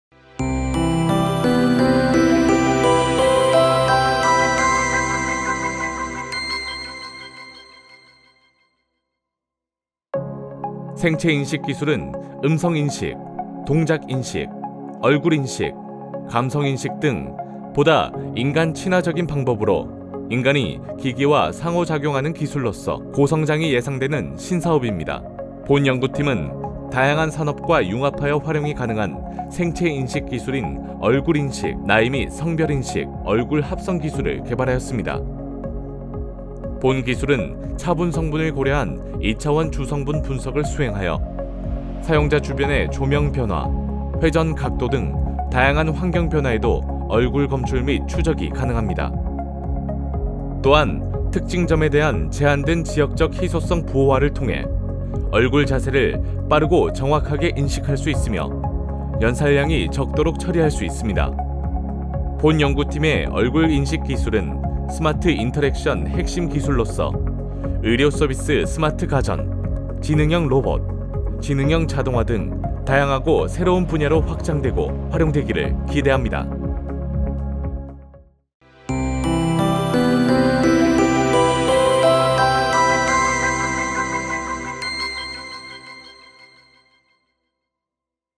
[테크블랙홀] 운전자용 생체인식 기술_대구경북과학기술원
다양한 산업과 융합하여 활용 가능한 생체인식 기술인 얼굴인식, 나이 빛 성별 인식, 얼굴 합성 기술을 개발